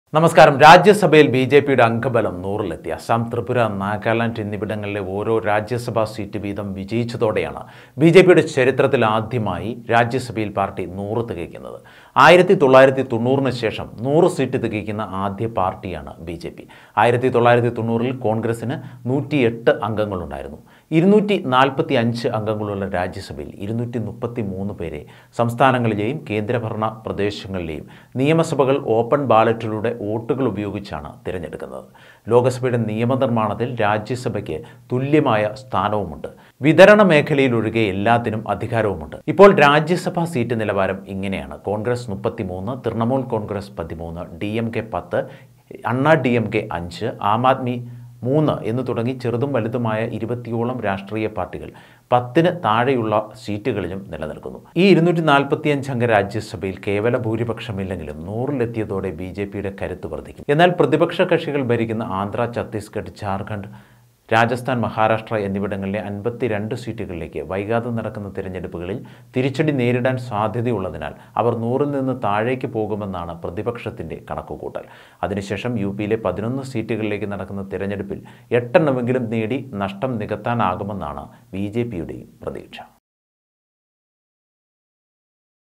രാജ്യസഭയില്‍ 100 സീറ്റ് തികച്ച് ബി ജെ പി l bjp touches 100 mark in rajya sabha
രാജ്യസഭയിൽ ബിജെപിയുടെ അംഗബലം നൂറിലെത്തി. അസം, ത്രിപുര, നാഗാലാൻഡ് എന്നിവിടങ്ങളിൽ ഓരോ രാജ്യസഭാ സീറ്റ് വീതം വിജയിച്ചതോടെയാണിത്. ബിജെപിയുടെ ചരിത്രത്തിൽ ആദ്യമായാണ് രാജ്യസഭയിൽ പാർട്ടി 100 തികയ്ക്കുന്നത്. 1990 നു ശേഷം 100 സീറ്റ് തികയ്ക്കുന്ന ആദ്യ പാർട്ടിയാണു ബിജെപി. 1990 ൽ കോൺഗ്രസിനു 108 അംഗങ്ങളുണ്ടായിരുന്നു.